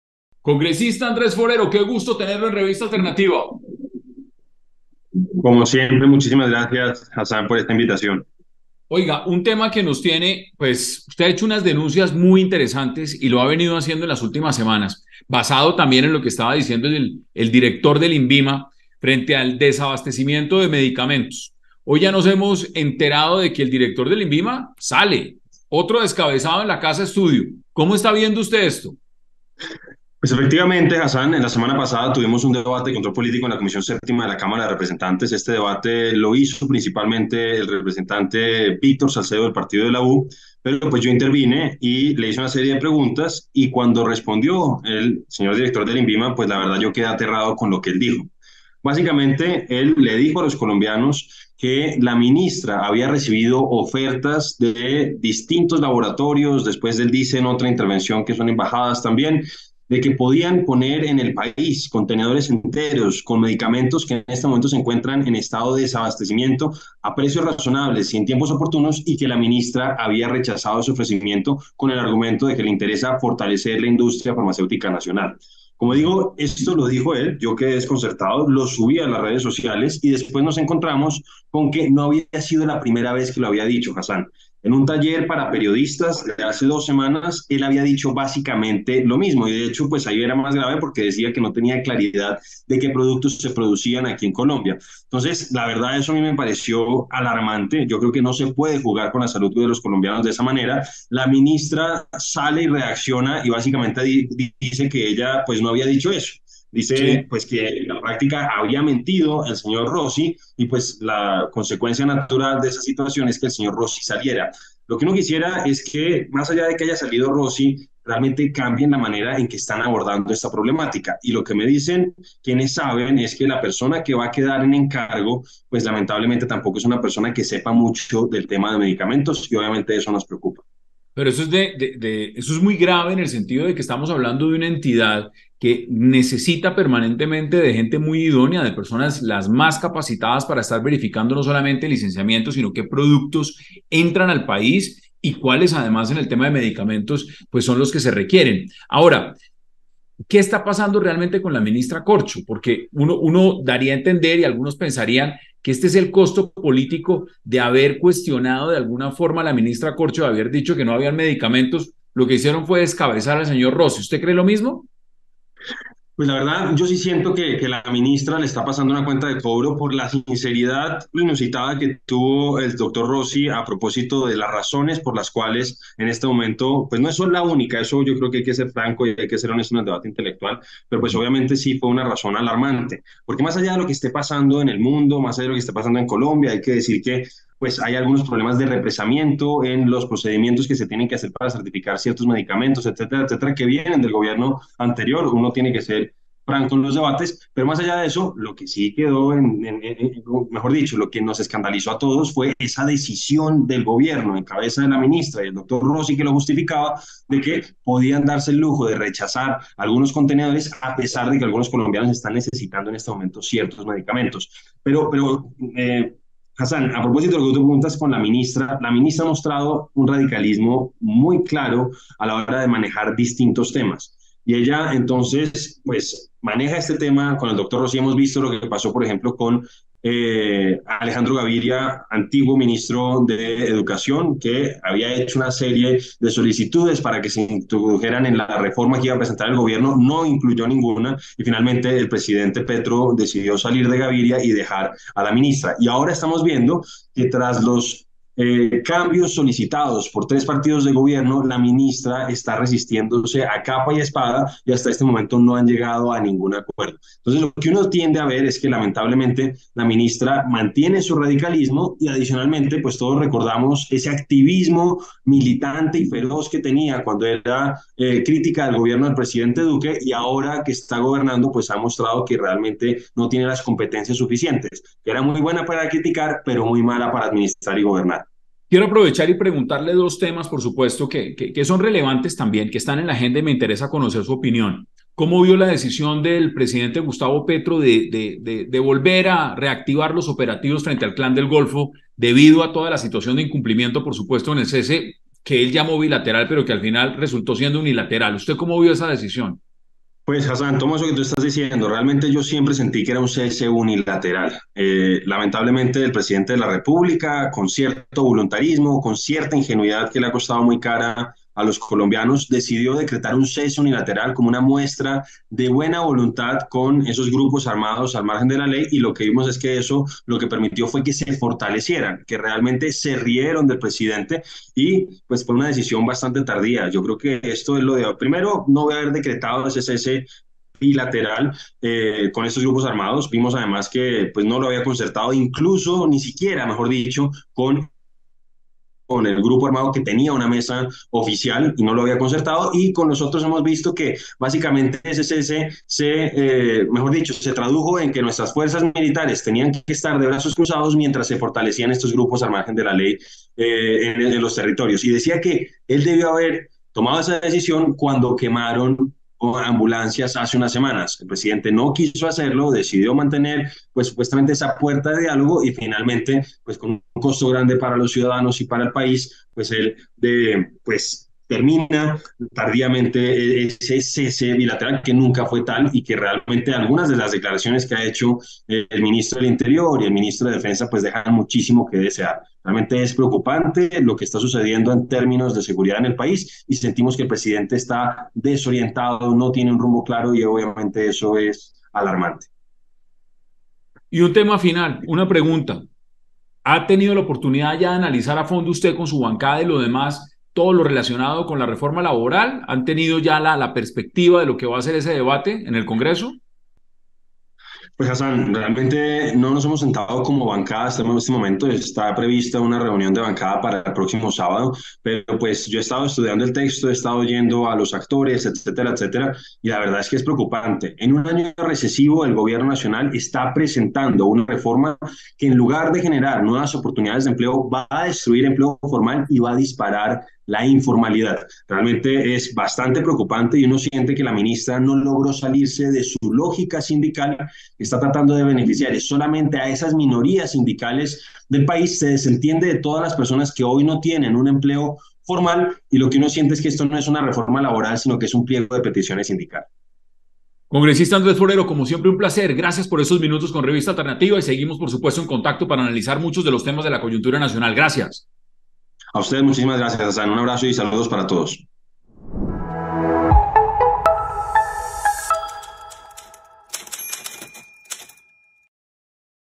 EntrevistaAlternativa - Invima sin director en medio de escasez de medicamentos - Andrés Forero
Andrés Forero, congresista del Centro Democrático habló con RevistaAlternativa acerca de la salida del director del Invima luego de hacer declaraciones sobre la escasez de medicamentos y la postura de la ministra Corcho al respecto.